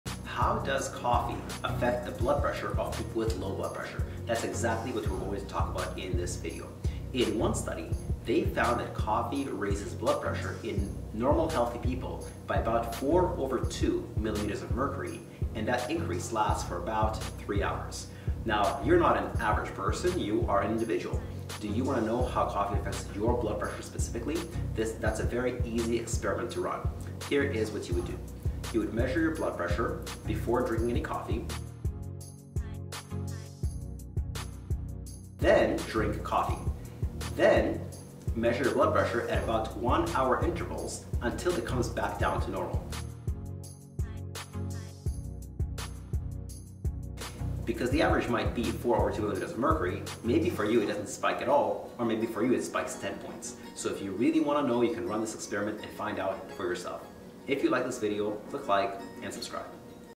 Coffee for Low Blood Pressure: Does it Work?
Learn:

• By how much does coffee raise blood pressure?
• How long does this increase last?
• A simple experiment you can run on yourself to understand by how much coffee raises your blood pressure.